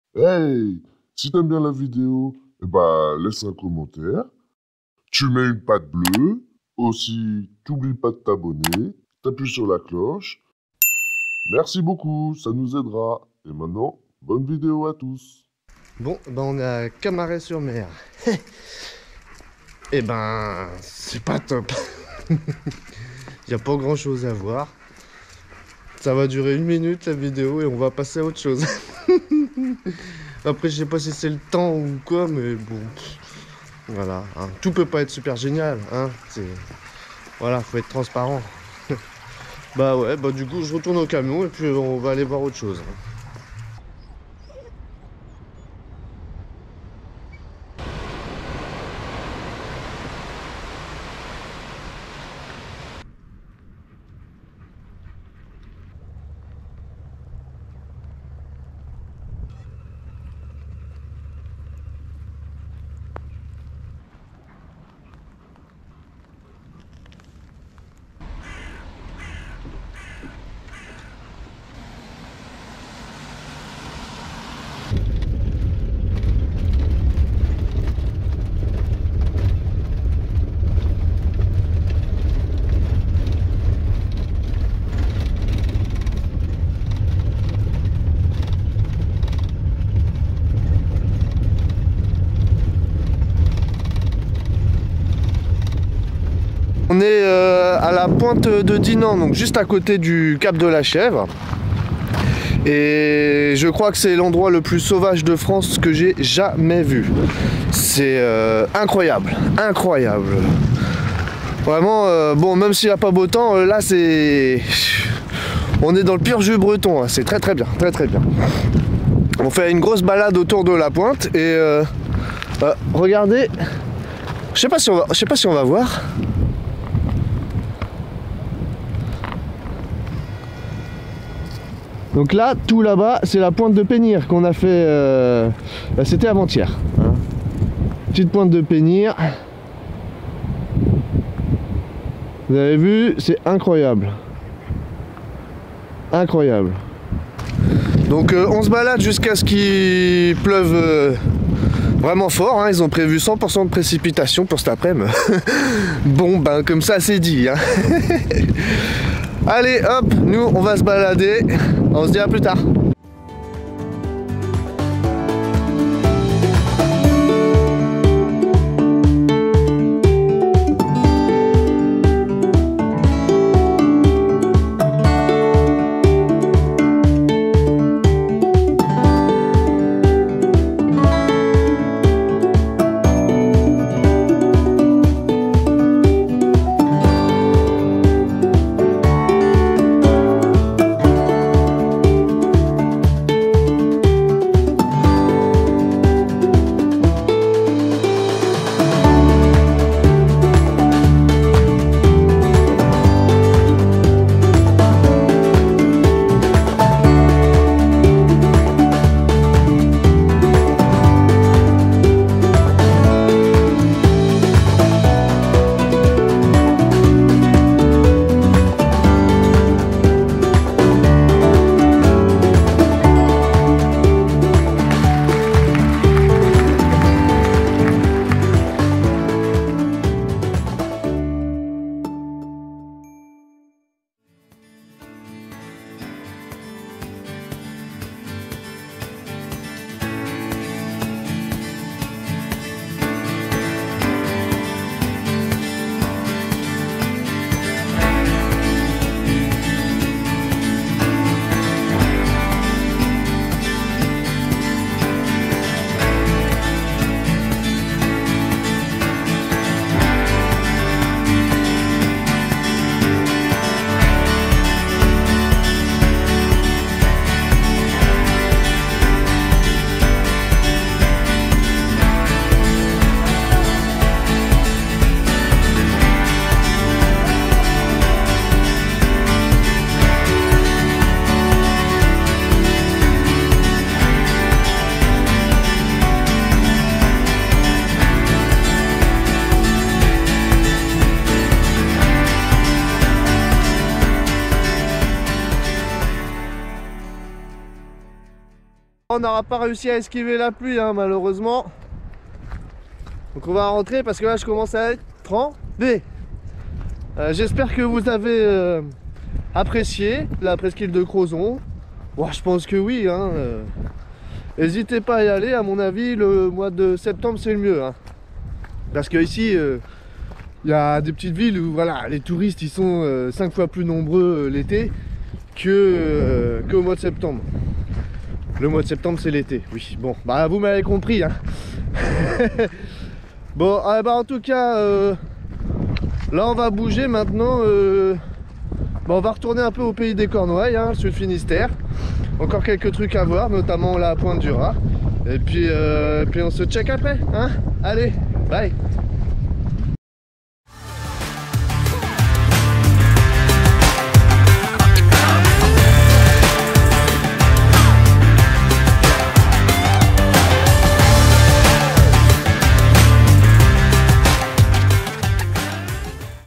On commence moyen pour finir bien (camaret-sur-mer / pointe de dinan )
Salut à tous ! J'espère que vous allez bien ?
Comme dit dans le titre on commence la vidéo par un coin pas super (ça arrive...) et on continu avec un endroit sauvage super .